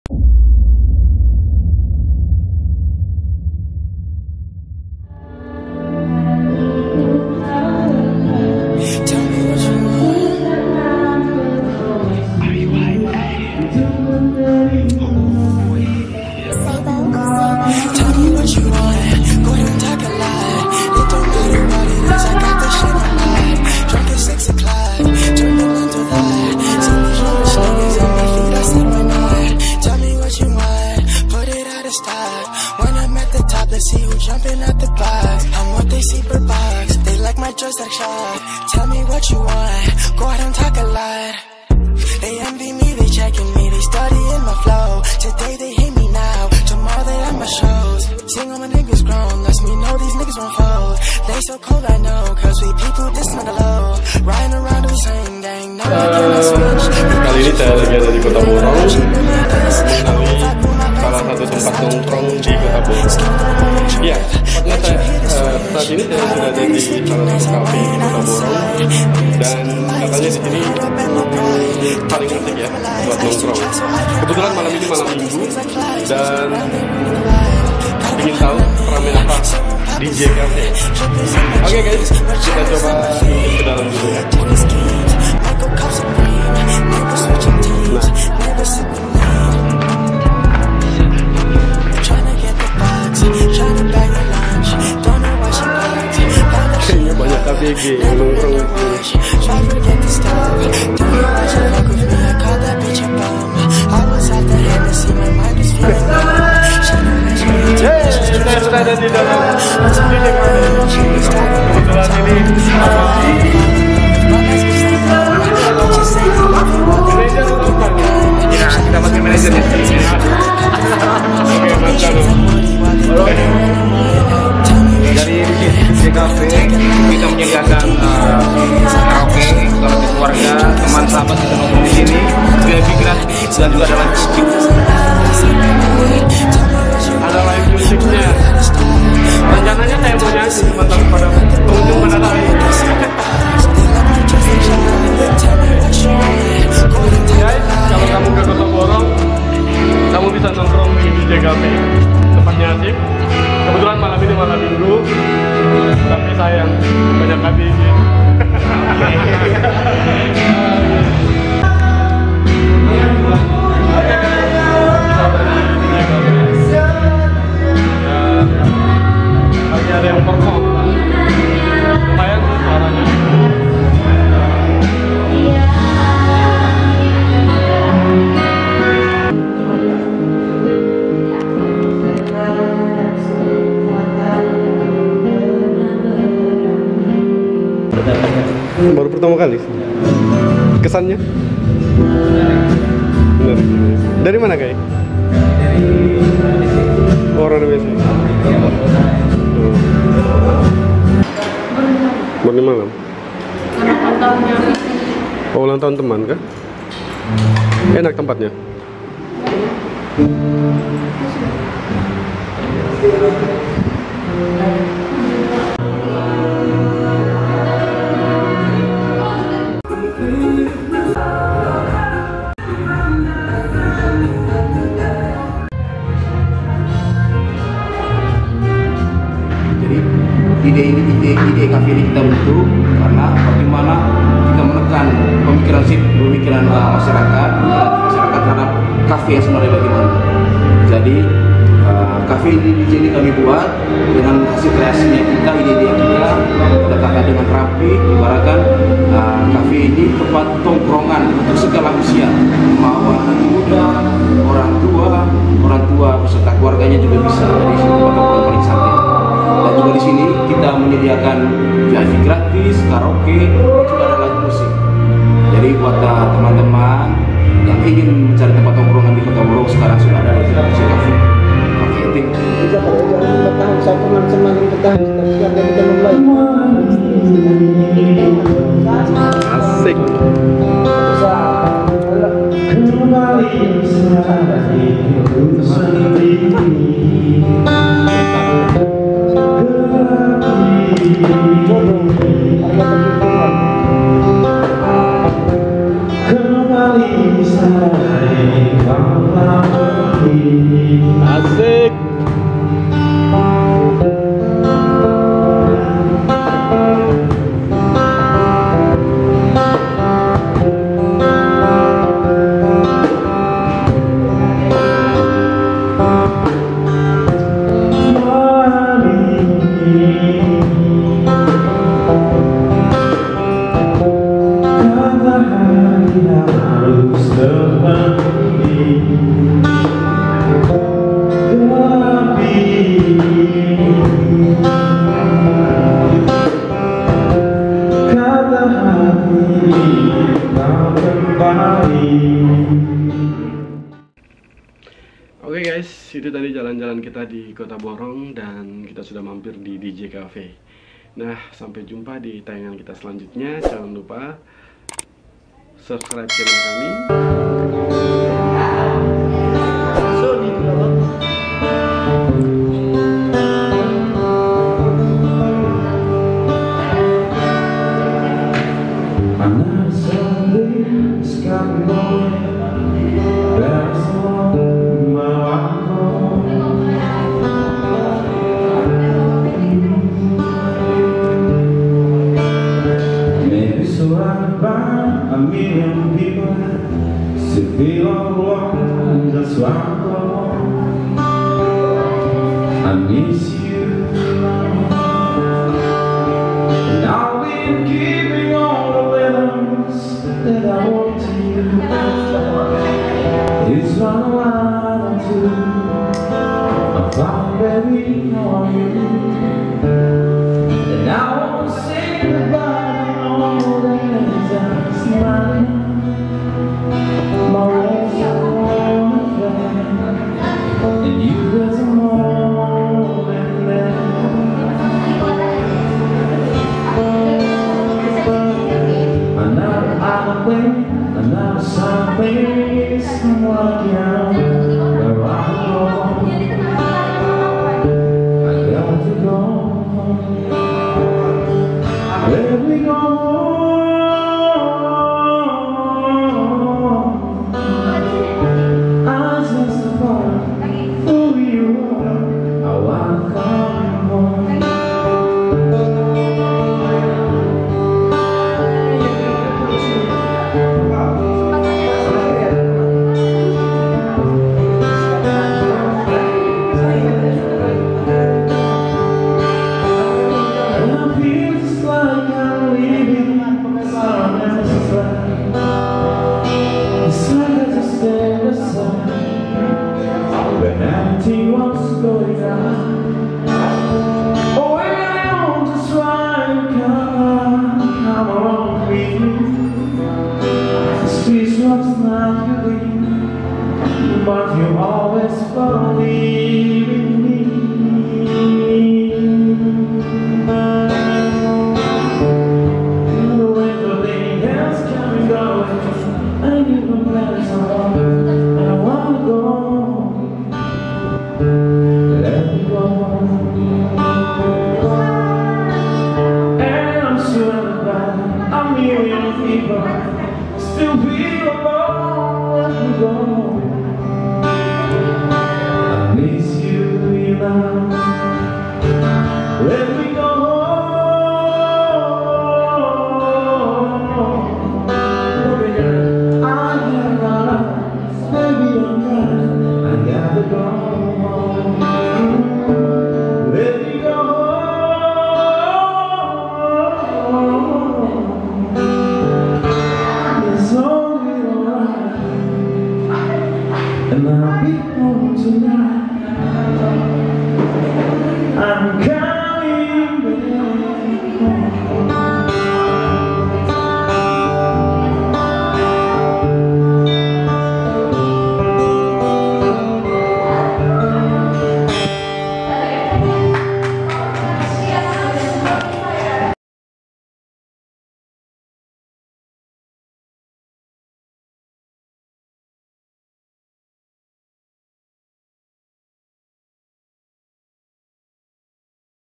DJ CAFFE TEMPAT ASYIEK BUAT NONGKRONG DI KOTA BORONG MANGGARAI TIMUR FLORES NTT
DJ CAFEE ATAU DELTA JAYA CAFE TERTLETAK DI KELURAHAN RANA LOBA KOTA BORONG MANGGARAI TIMUR.TEMPAT YANG ASYIEK BUAT NONGKRONG UNTUK SEMUA USIA..DISINI ADA WIFI/COFEEBREAK/LIVEMUSIC/KARAOKE/DLL
CAFE INI BARU DIIDIRIKAN PADA AWAL DESEMBER 2018